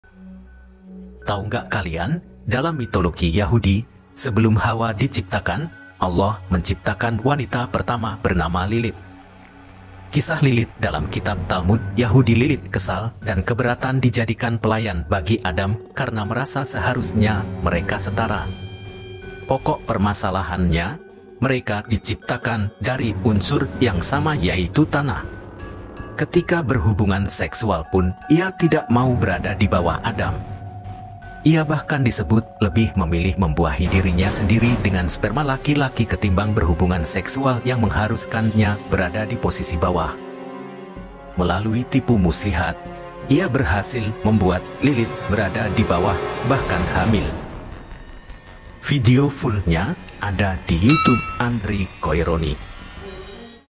LEGENDA LILITH, ISTRI PERTAMA ADAM DALAM MITOLOGI YAHUDI
Dalam mitologi yahudi, Konon, sebelum Hawa, Adam sempat menikahi seorang perempuan yang diciptakan Tuhan dari tanah. Setidaknya, itu yang terdapat pada folklor Yahudi. Perempuan itu bernama Lilith. Sosok ini berasal dari kata Sumeria “lilitu” yang berarti roh angin atau iblis perempuan. Barangkali itu sebabnya ia dianggap sebagai perempuan paling berbahaya dan misterius di dunia.
Kisah tentang Lilith memang sangat kontroversi. Dikisahkan ia enggan dianggap sebagai second sex karena sama-sama diciptakan dari tanah. Yap, kisah tentang Lilith tidak jauh-jauh dari penolakan superioritas kaum Adam. Hm, baru tahu kan kalau kesetaraan gender sudah disinggung sejak abad-7 ?.

Kisah Lilith Dalam Kitab Talmud Yahudi

Lilith kesal dan keberatan dijadikan pelayan bagi Adam karena merasa seharusnya mereka setara. Pokok permasalahannya, mereka diciptakan dari unsur yang sama yaitu tanah. Ketika berhubungan seksual pun ia tidak mau berada di bawah Adam.
Ia bahkan disebut lebih memilih membuahi dirinya sendiri dengan sperma laki-laki ketimbang berhubungan seksual yang mengharuskannya berada di posisi bawah.
Adam yang merasa ditentang tidak tinggal diam. Melalui tipu muslihat, ia berhasil membuat Lilith berada di bawah bahkan hamil. Lilith yang marah kemudian angkat kaki dari surga dan tinggal di Laut Merah.
Kesepian karena ditinggal istri pergi, Adam berkeluh kesah pada Tuhan. Kemudian diutuslah tiga malaikat Snvi, Snsvi, dan Smnglof untuk membawa Lilith kembali. Sayangnya, Lilith yang kadung sakit hati enggan pulang. Ketiga malaikat itu bahkan mengancam akan membunuh satu persatu anak Lilith jika ia tidak mau menurut.

“Tidak tahukah kalian jika aku diciptakan untuk membawa penyakit mematikan pada anak-anak manusia. Setiap bayi laki-laki yang dilahirkan, aku berkuasa atas hidupnya selama 8 hari dan untuk bayi perempuan aku berkuasa atas dirinya selama 20 hari,” kata Lilith angkuh.
“Jadi, kalau kalian ingin membunuhnya, lakukan saja sekarang. Itu malah meringankan tugasku,” lanjutnya.
Mendengar kata-kata Lilith, ketiga malaikat itu akhirnya tidak bisa berbuat apa-apa. Mereka kembali ke surga dengan tangan hampa. Mengetahui istrinya enggan kembali, Adam kembali berkeluh kesah mengenai kesepiannya kepada Tuhan. Untuk Adam, Tuhan menciptakan Hawa dari tulang rusuknya.